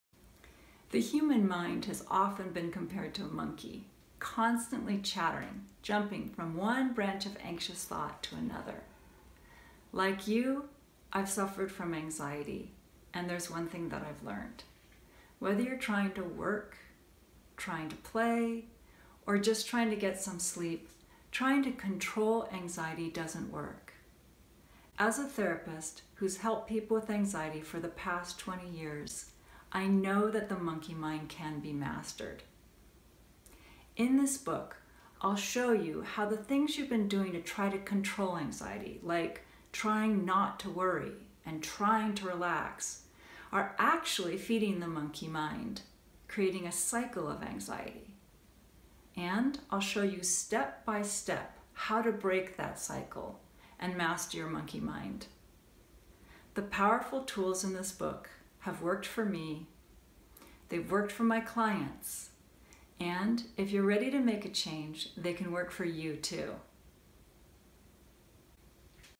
Don't Feed the Monkey Mind
The author's introduction to her book about stopping the cycle of anxiety and worry.